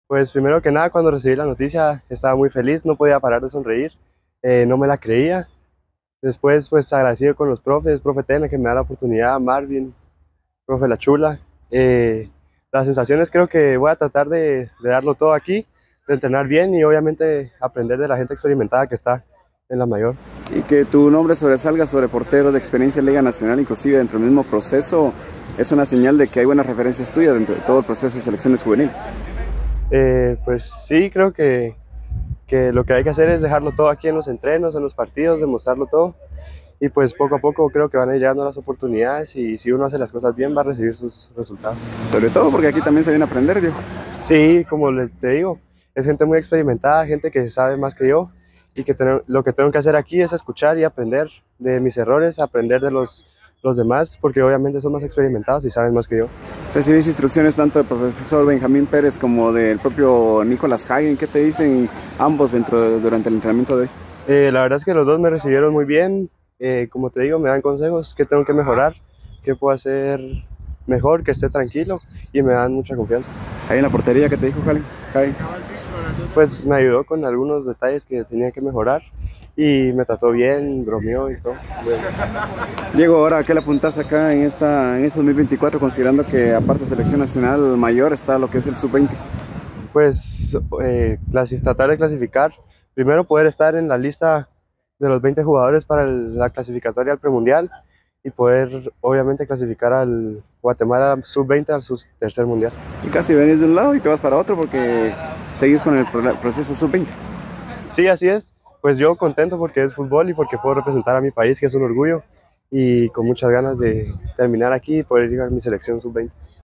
Diego Bolaños sobre su convocatoria a Selección de Guatemala vs Islandia
El portero de 17 años fue convocado por Luis Fernando Tena para el partido del sábado de la selecciondeguatemala vs Islandia ante la lesión del arquero de comunicacionesfc Fredy Pérez.